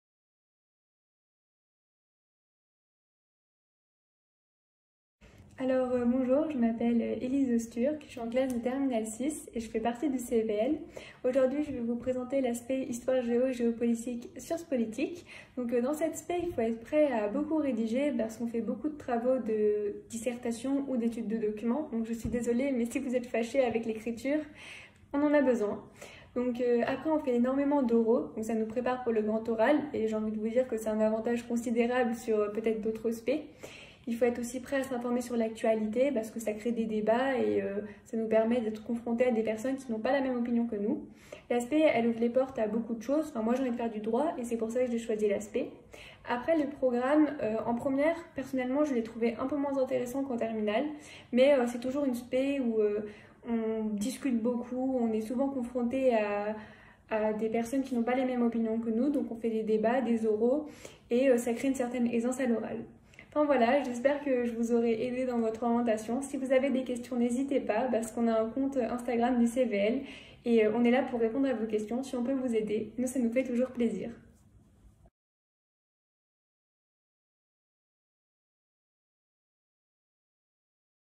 La spécialité Histoire Géographie Géopolitique Sciences Politiques du baccalauréat général
Vidéo réalisée par les élèves du CVL et de la MDL du lycée Louis Armand - janvier 2021